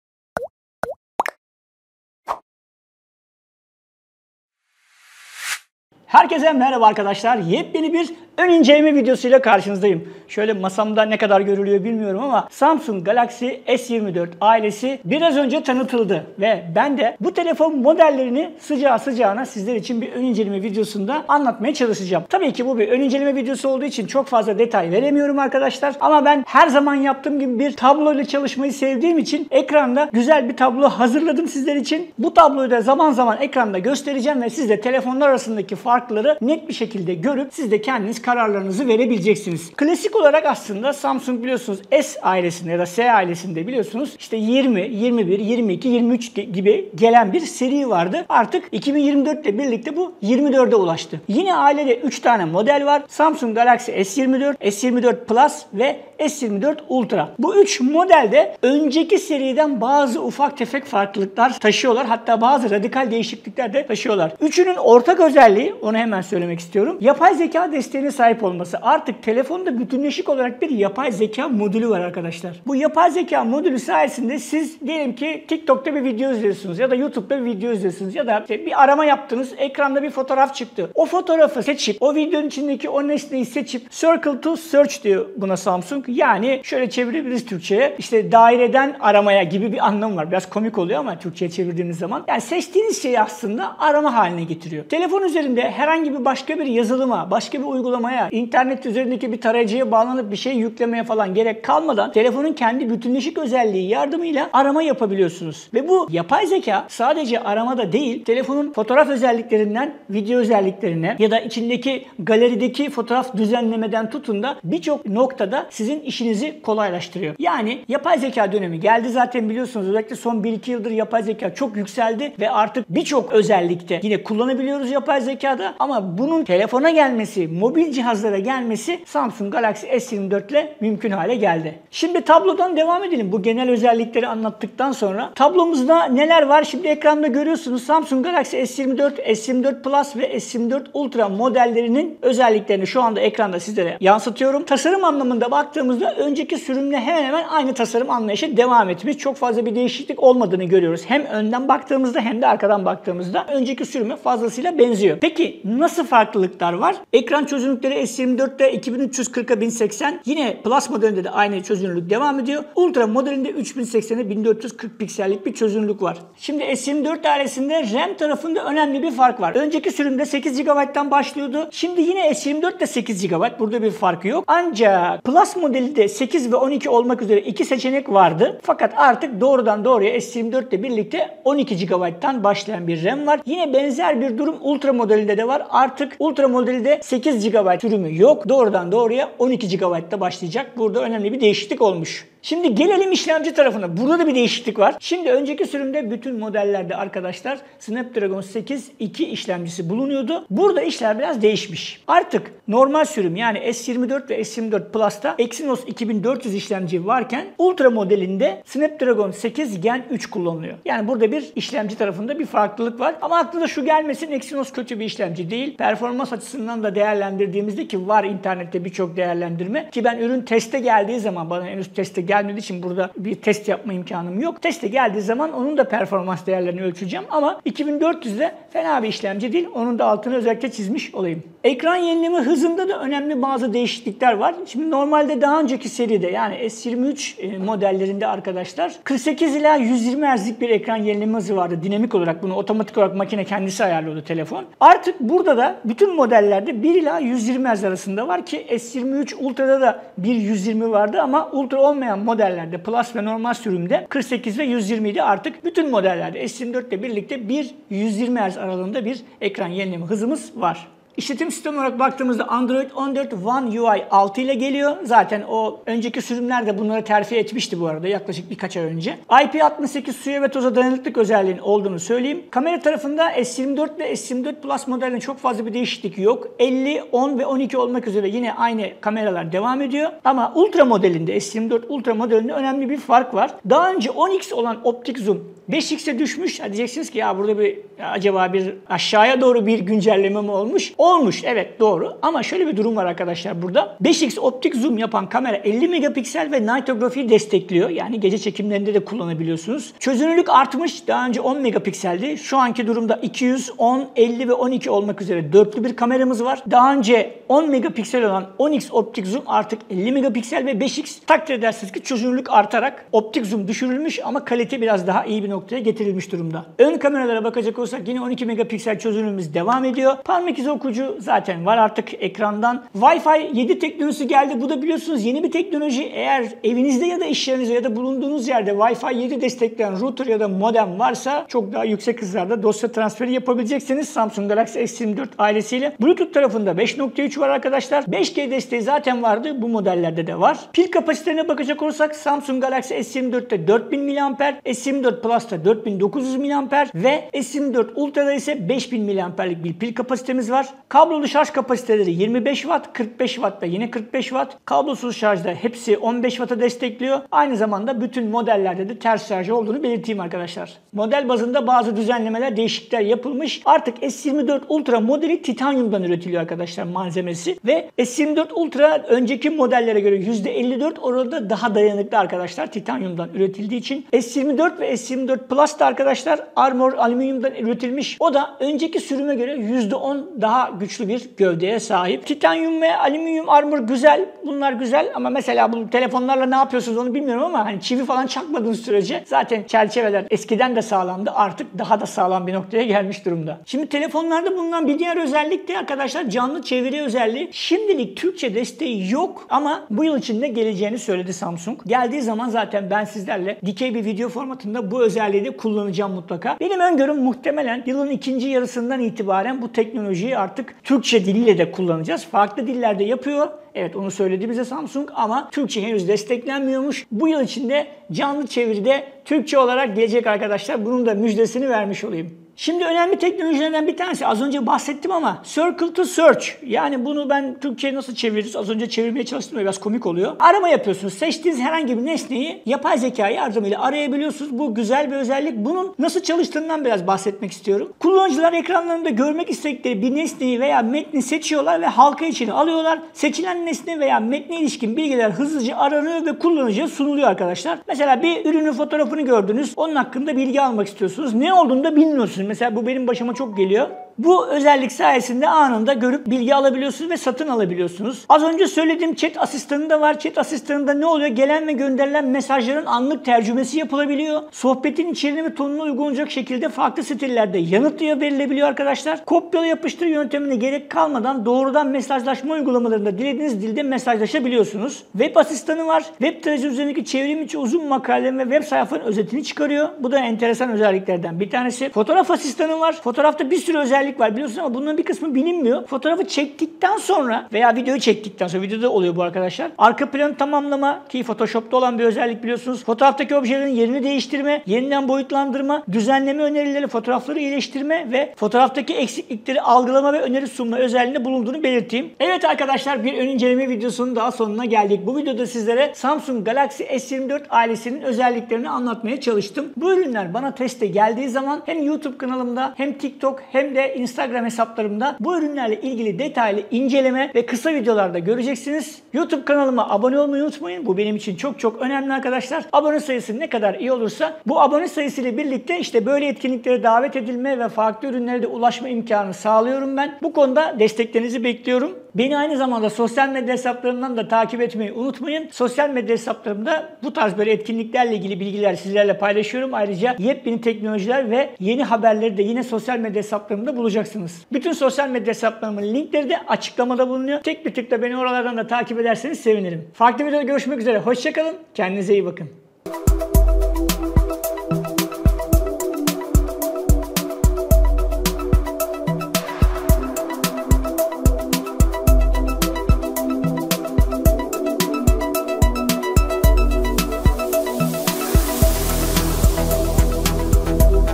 Samsung Galaxy S24 serisi ile yapay zeka telefona geldi!
Samsung Galaxy S24 cep telefonu ailesi tanıtıldı ve ben de sıcağı sıcağına sizlere bir ön inceleme videosu hazırladım. Bu videoda telefonların genel özelliklerine ve yapay zeka ile sunulan fonksiyonlarına göz attım.

S24 ailesinde klasik olarak Samsung Galaxy S24, S24 Plus ve S24 Ultra modelleri bulunuyor. Bu modellerin önceki sürümden önemli bir farkı ise artık S24 ve S24 Plus modellerinde Exynos 2400 işlemci kullanılırken, S24 Ultra modelinde Snapdragon 8 Gen 3 işlemci olması. Yani artık Ultra dışındaki modellerde Exynos işlemci kullanılacak.